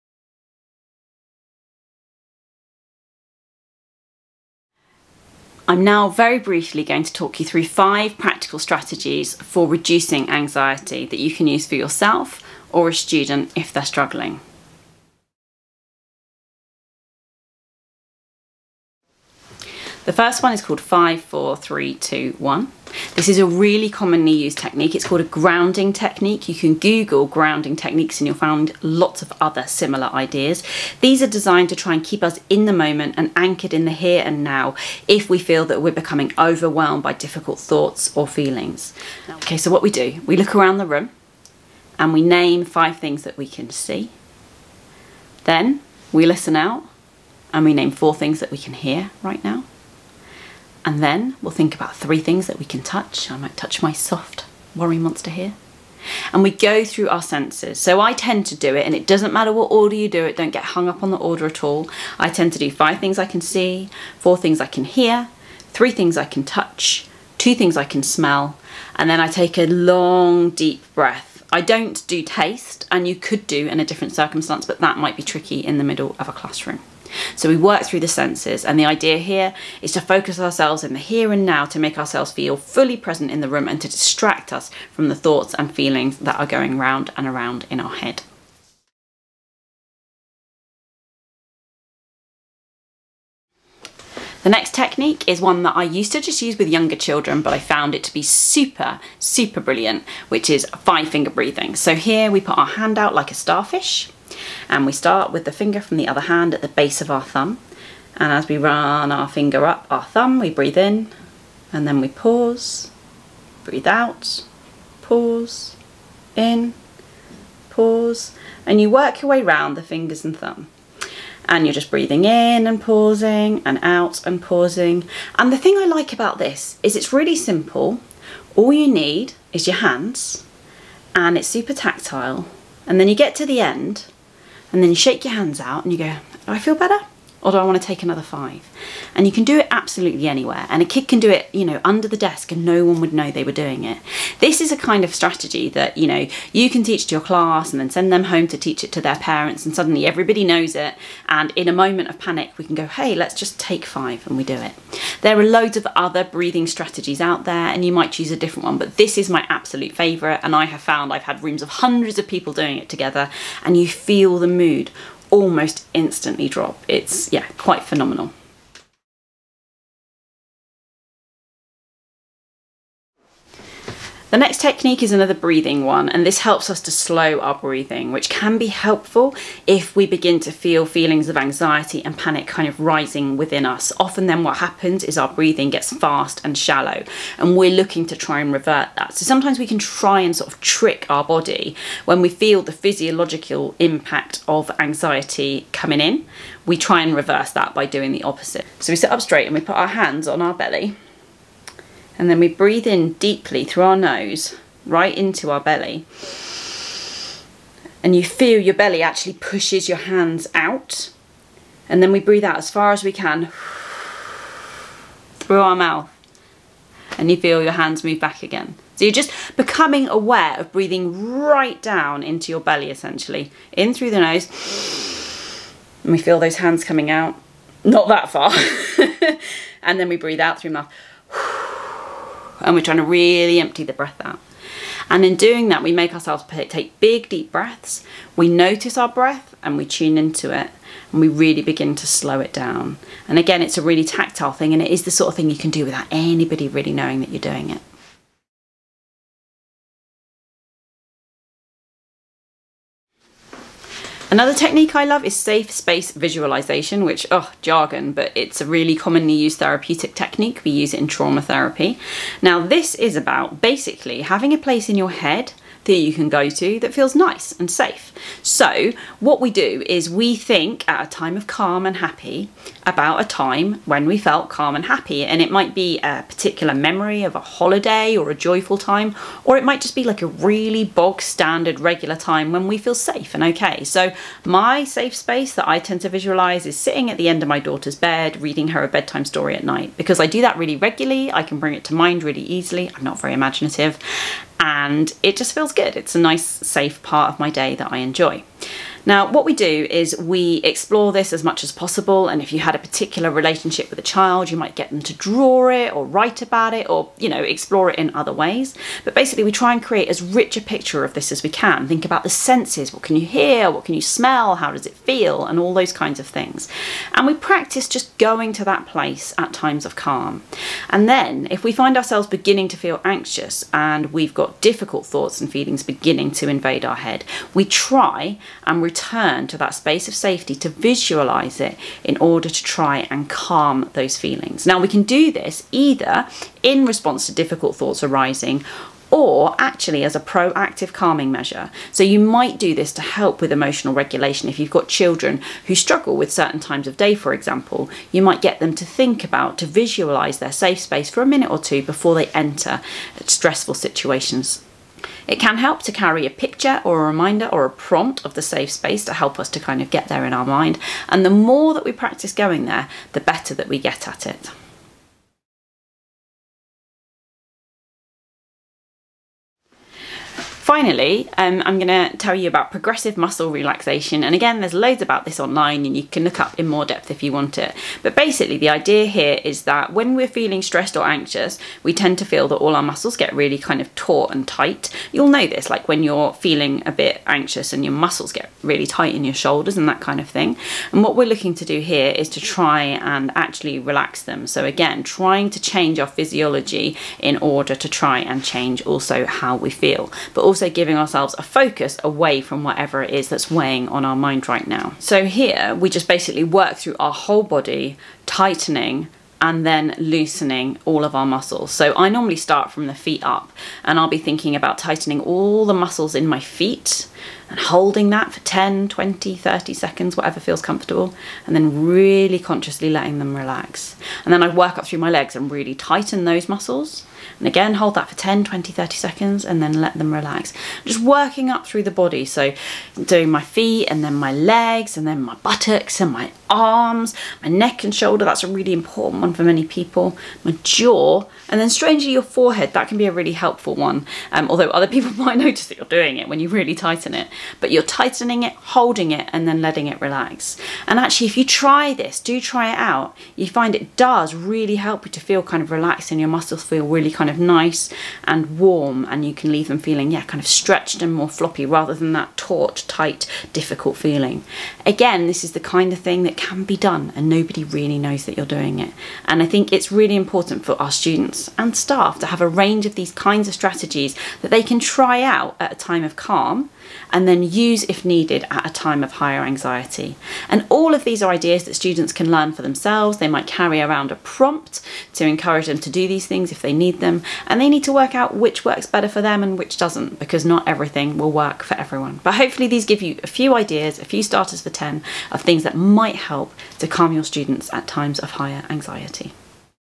ANXIETY | 5 Simple Strategies for Managing Anxiety
In this video, I share five simple strategies that can be used by adults or children to manage anxious thoughts and feelings.  These ideas are all ones you can use t almost anywhere and nobody even need know you're doing them - so they can be great for busy classroom situations for example.

There are some old favourites and a couple I've not shared before.  You can jump to individual ideas using the timestamps below or video chapters - or you can watch the whole thing. I hoped it might be helpful for INSET etc - please feel free to use and share it as you wish and let me know if it was helpful.

00:00 - Introduction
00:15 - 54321
01:29 - 5 finger breathing
02:50 - belly breathing
04:09 - safe space visualisation
06:06 - progressive muscle relaxation

This is a video version of an article I wrote for SecEd.